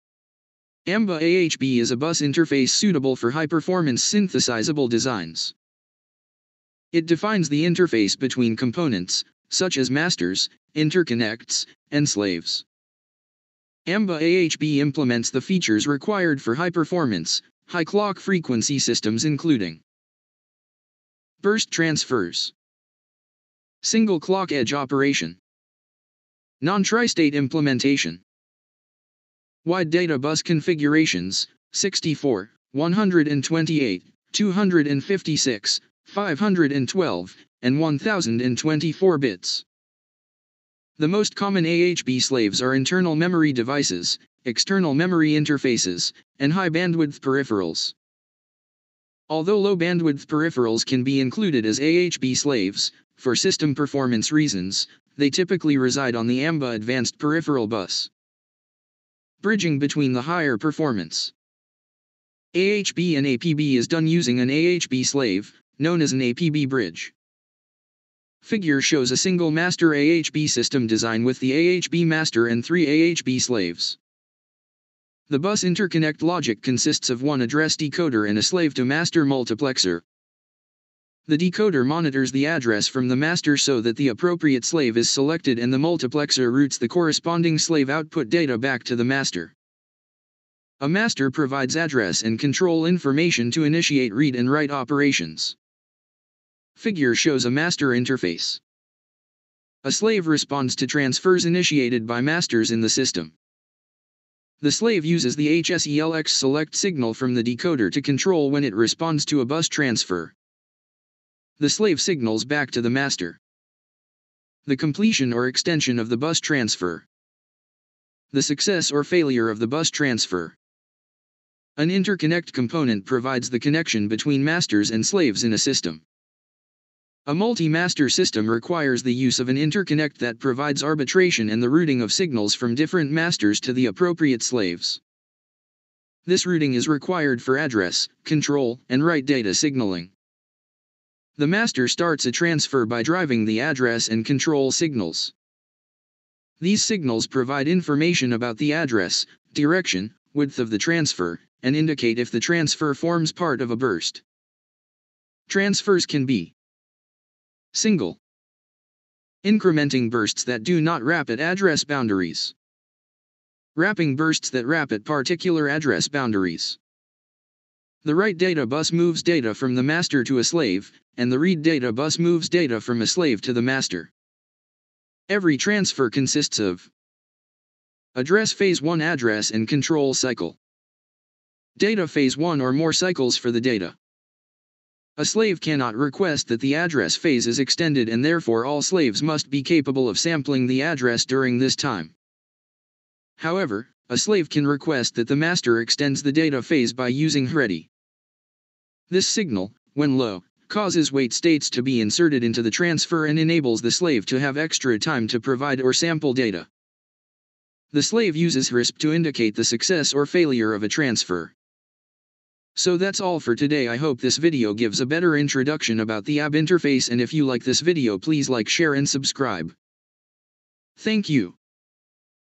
INTRODUCTION TO AHB INTERFACE || AMBA PROTOCOL || PART 1
This video is an introduction to amba ahb protocol and it discuss breifly  about ahb protocol.